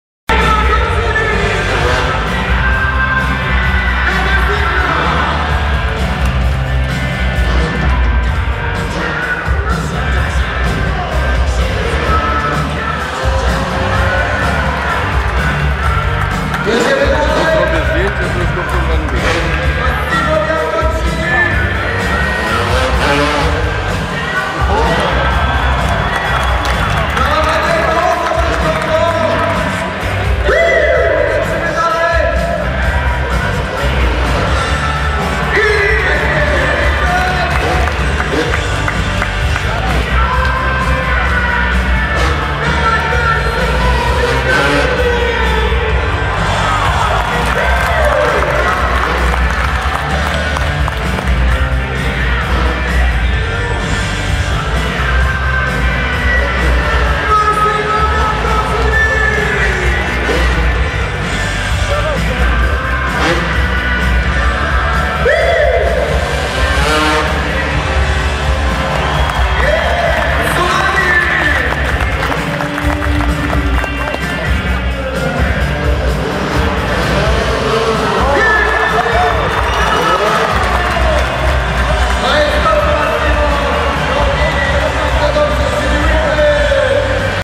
Night of The Jumps Gdansk/Sopot Ergo Arena
Mistrzostwa w motocrosie FMX Night of the jumps - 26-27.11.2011 FMX Night of the jumps motocross Sopot poland